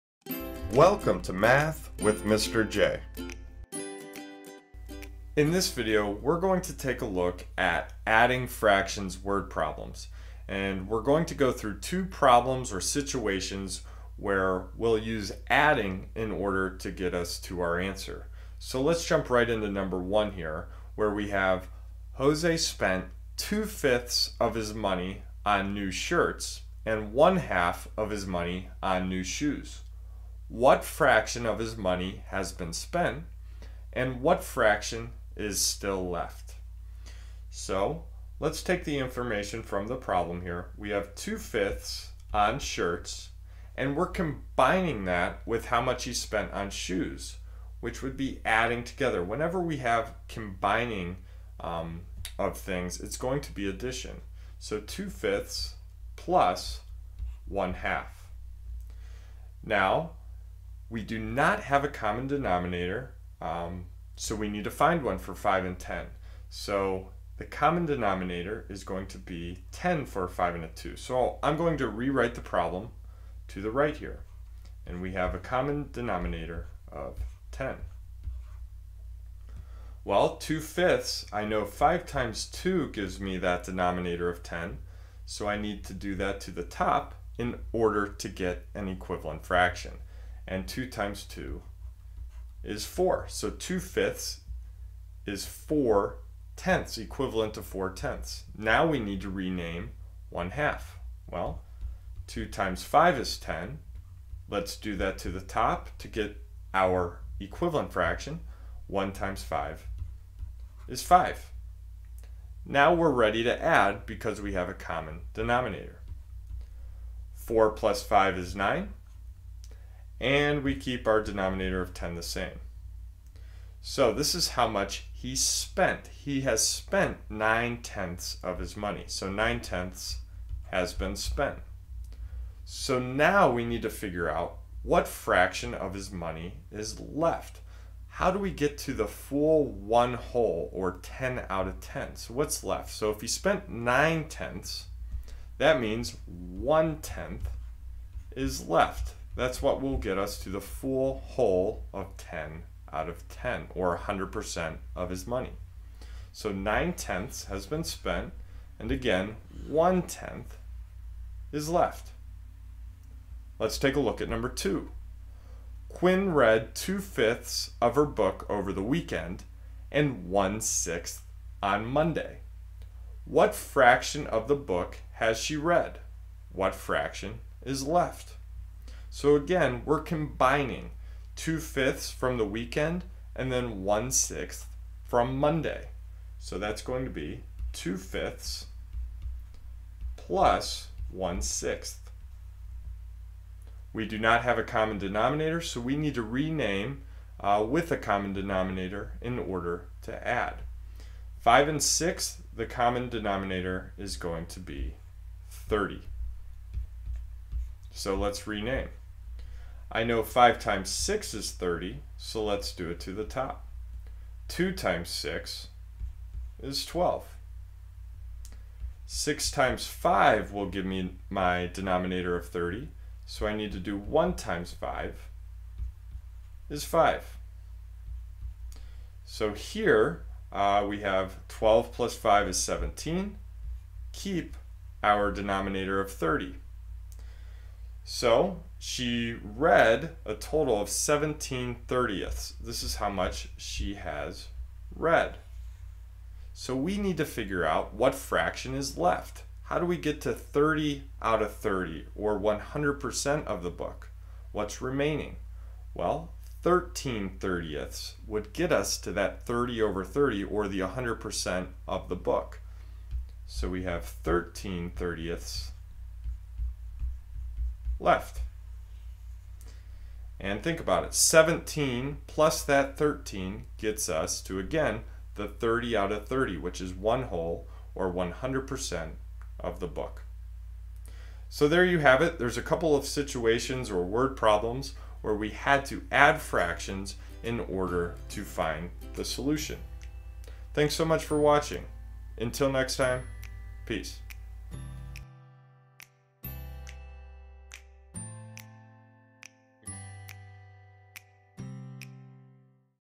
Adding Fractions Word Problems | Fraction Word Problems
Welcome to Adding Fractions Word Problems with Mr. J! Need help with adding fractions story problems? You're in the right place!

Whether you're just starting out, or need a quick refresher, this is the video for you if you're looking for how to solve fraction word problems. Mr. J will go through examples and explain how to solve fraction story problems.

Links to other types of word problems are below.

Hopefully this video is what you're looking for when it comes to adding fractions story problems.